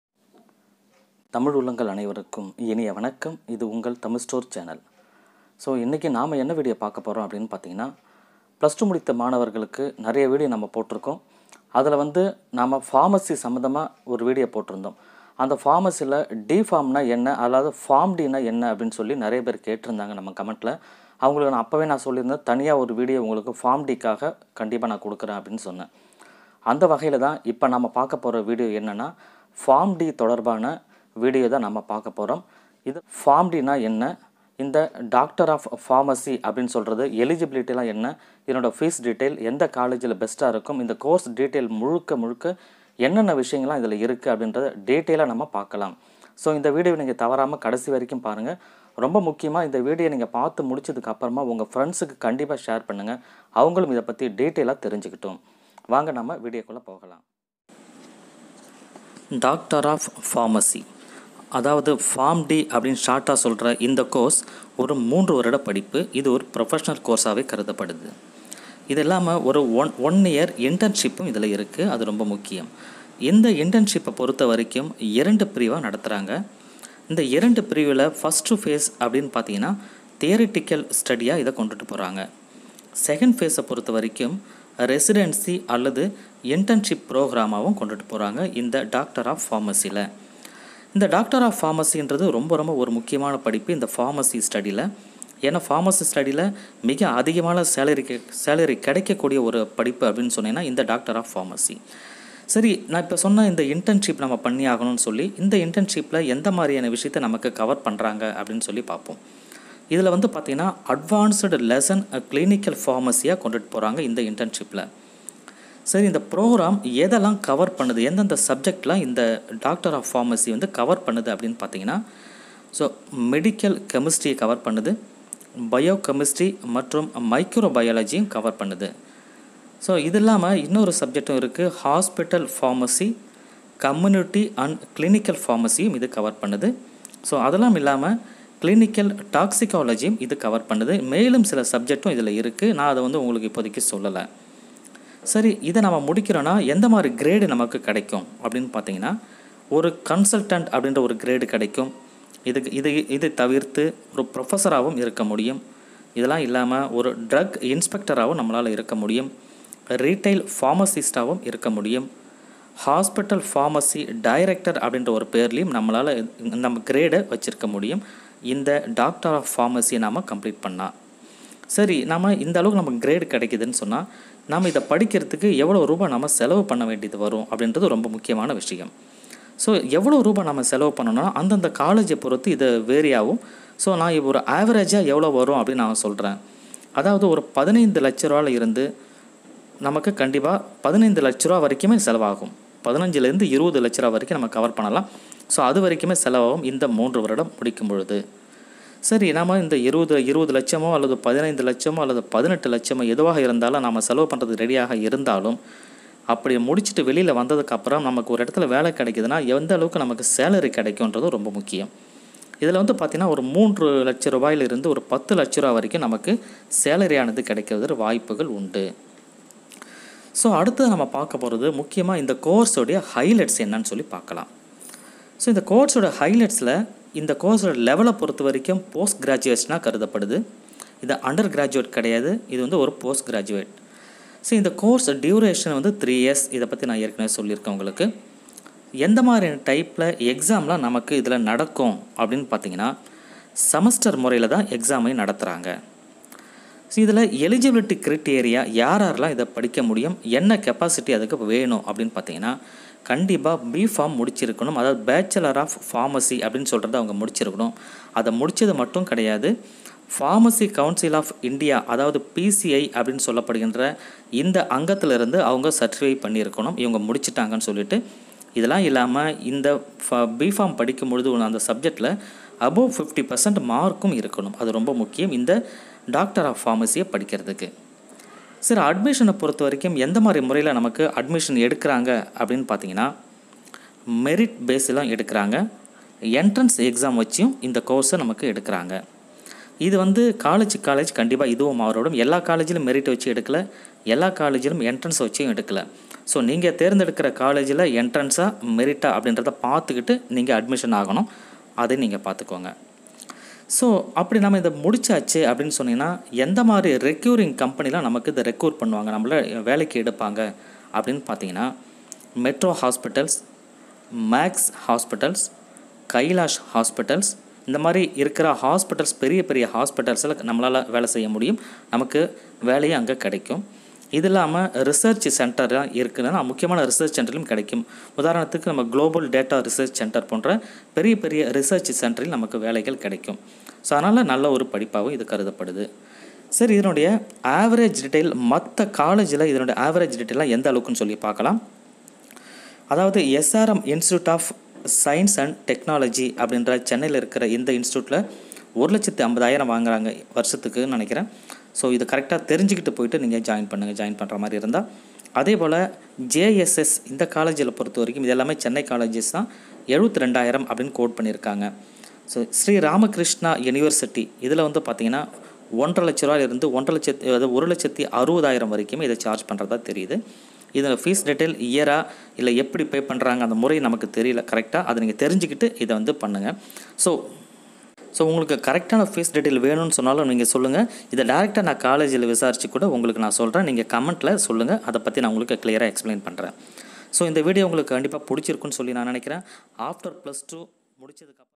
Pharm D course details and Eligibility cum Fees | Tamil store | in Tamil
This video guide to post graduate course of Pharm d, we are discuss about Eligibility, Fees details, Scope of this course,